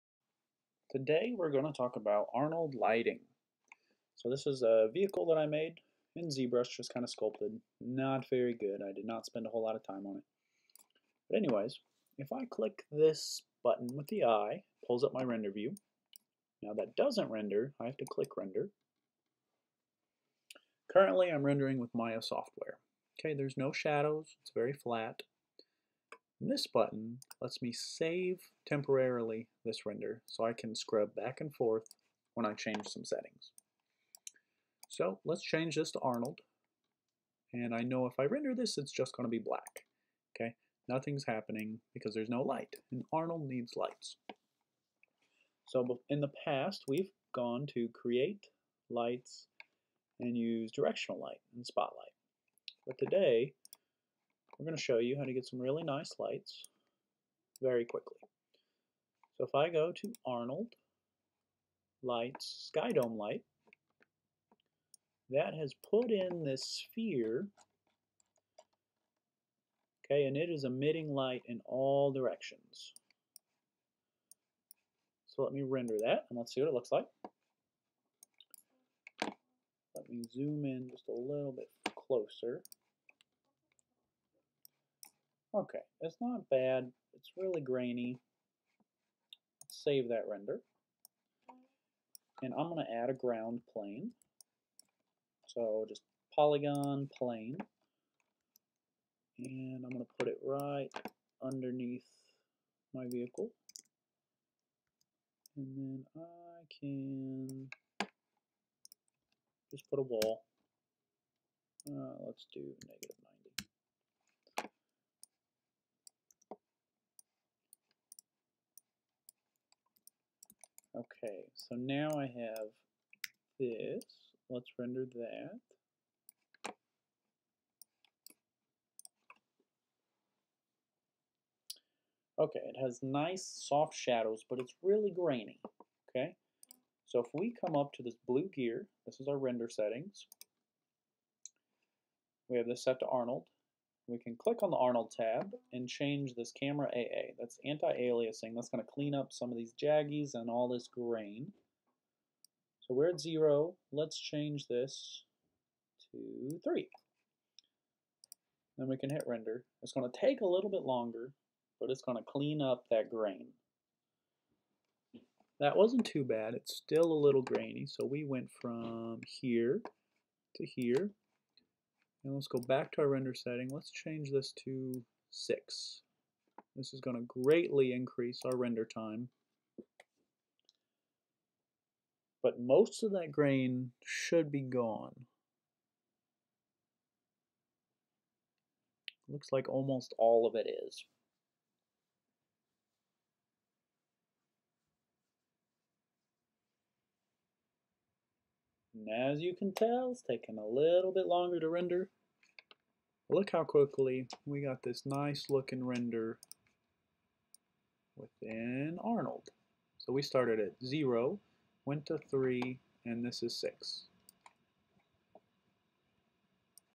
Arnold Skydome Light
A quick way to get decent lighting using an Arnold Skydome Light

Gear I recommend:
🖊 Drawing tablet I've Used for 10+ years